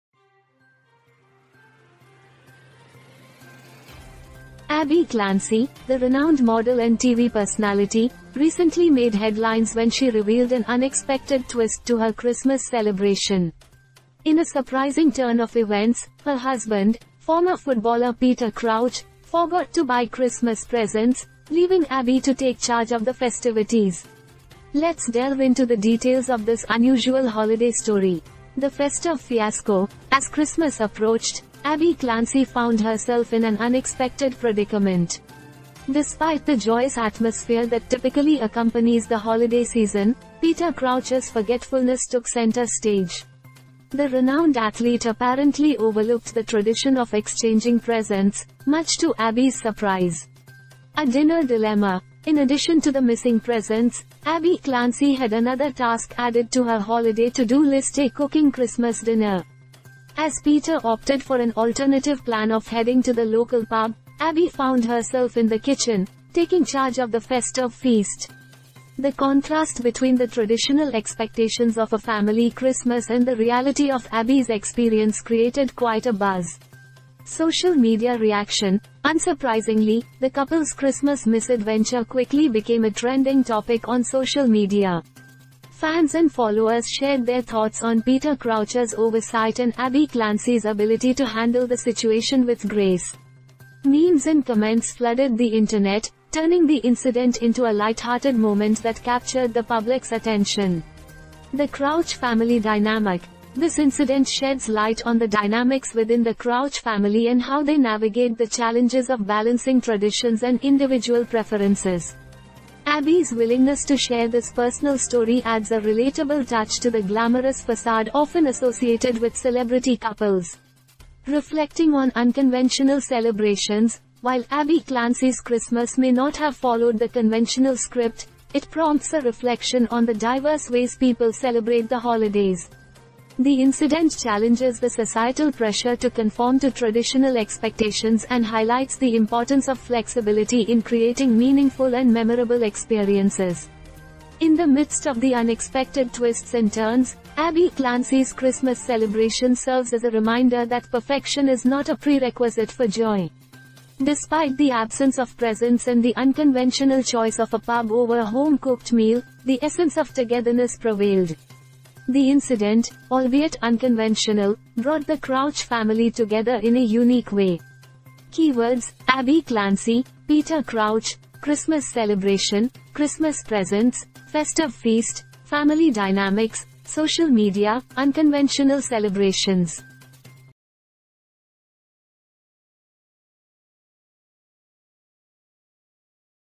Abbey Clancy's Christmas Surprise: When Peter Crouch Forgot the Presents
00:10 Abbey Clancy's Christmas Surprise: When Peter Crouch Forgot the Presents